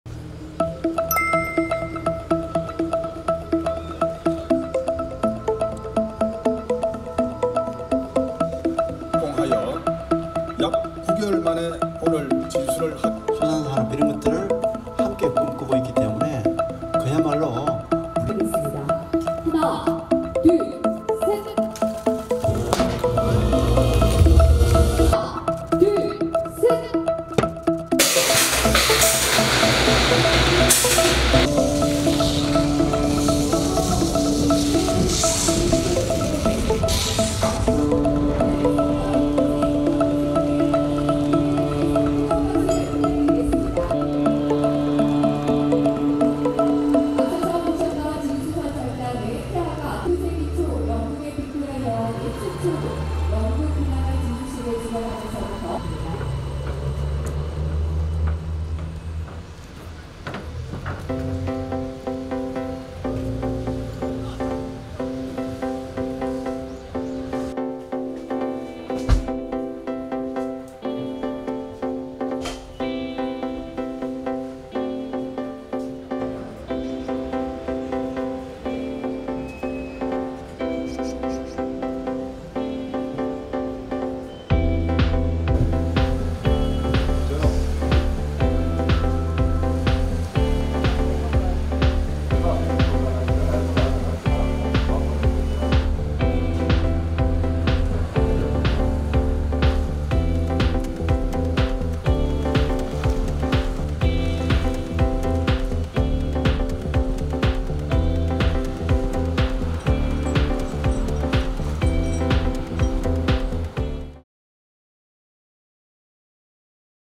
포항시, 다목적행정선 '연오세오호' 진수 | 영남일보
포항시는 29일 경남 거제시 소재 (주)아시아조선에서 다목적 행정선 '연오세오호' 진수식을 가졌다. 130t급의 중형 관공선인 '연오세오호'는 현재 운용 중인 29t급 소형 어업지도선인 '경북 207호' 대체 선박으로, 전국 기초자치단체 중에서는 가장 큰 규모다.
대구·경북지역 종합일간지 '영남일보'